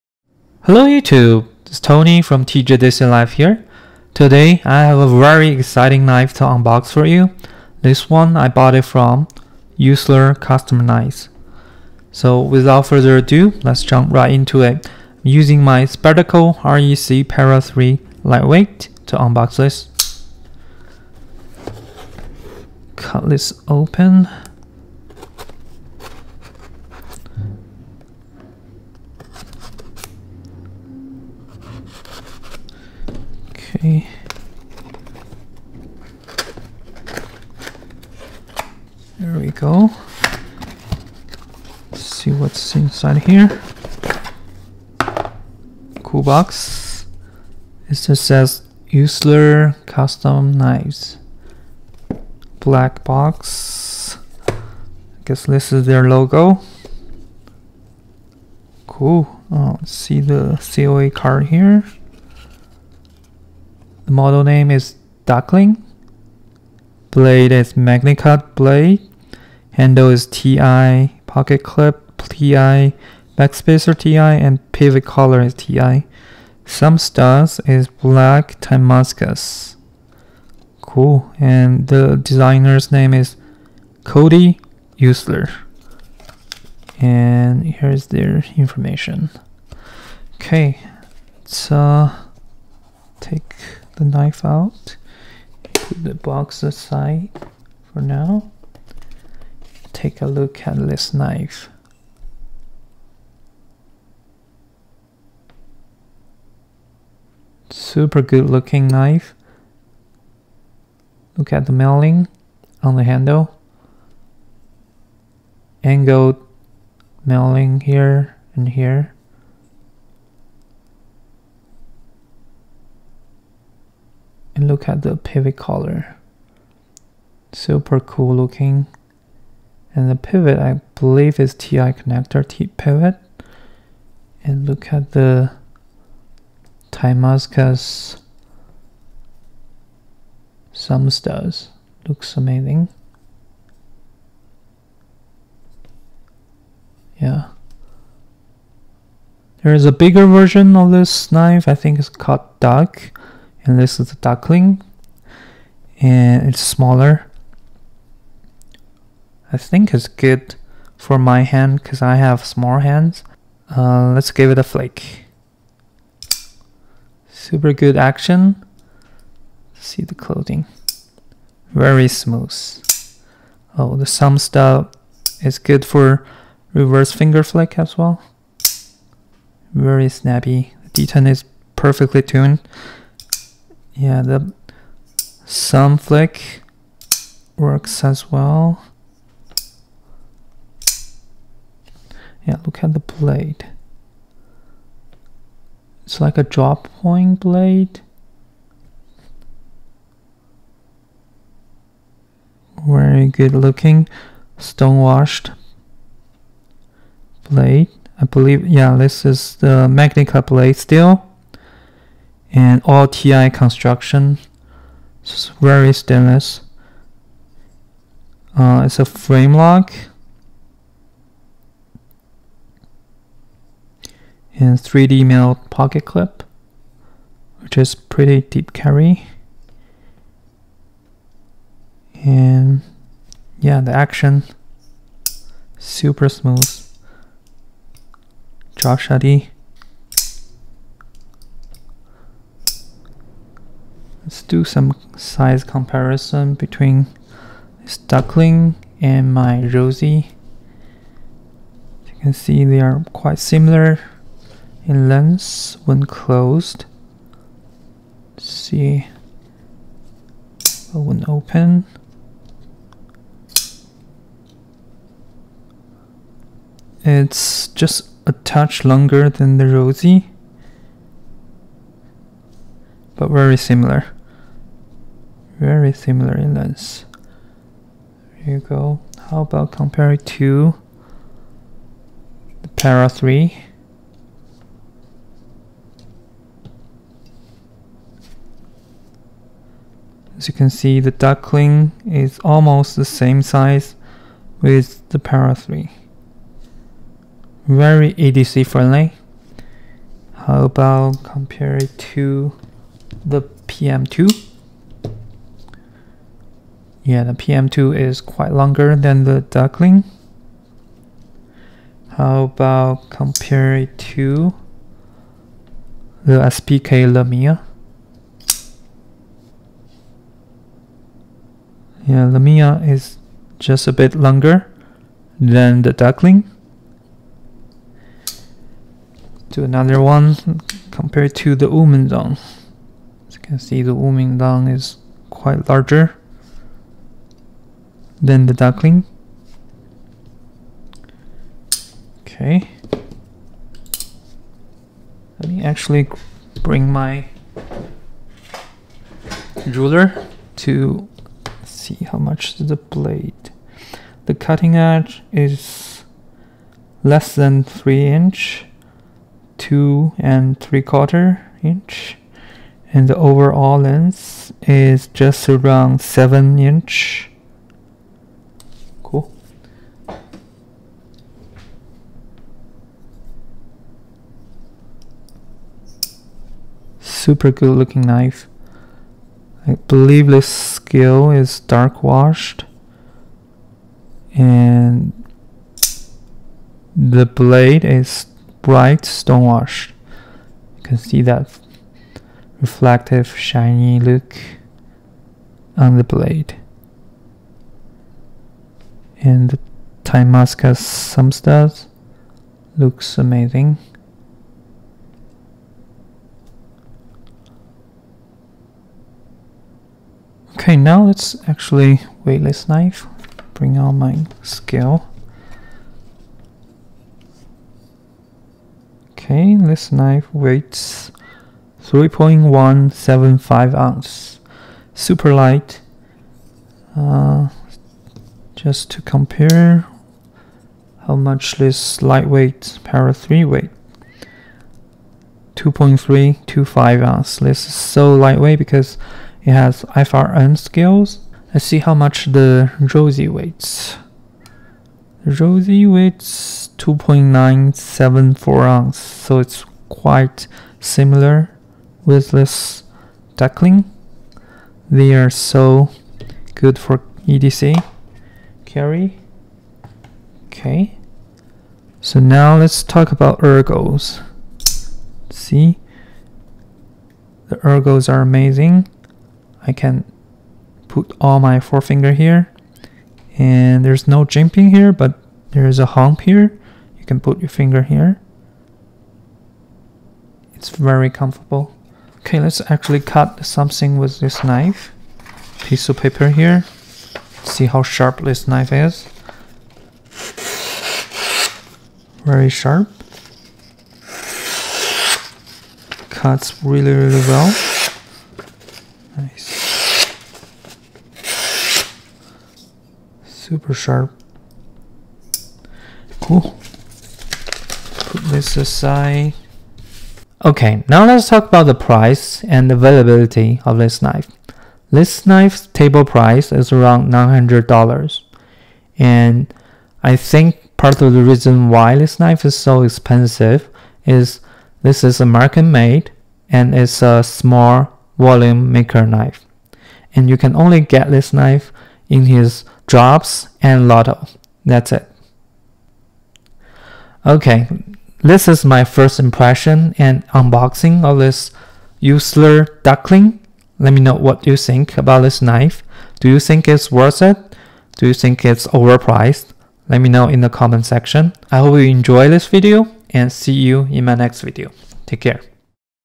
Eutsler Dukling Unboxing & First Impressions | Is This $900 EDC Knife Worth It? TJ EDC Life
In this video, we’re unboxing the Eutsler Dukling, a $900 premium EDC knife. We’ll share our first impressions, explore the craftsmanship, and discuss whether this high-end knife lives up to the price tag. If you’re into luxury knives or looking for the best EDC knife in the premium category, this is a must-watch.
🗡️ Eutsler Dukling Overview:

Blade Material: Magnacut
Handle: Titanium
Price: $900

🔎 What Makes a $900 EDC Knife Special?

Handcrafted details and premium materials
Limited edition release

00:00 – Introduction
01:10 – Unboxing the Eutsler Dukling
03:45 – First Impressions of the Design and Materials
06:30 – Features and Craftsmanship Breakdown
08:30– Is This $900 Knife Worth It?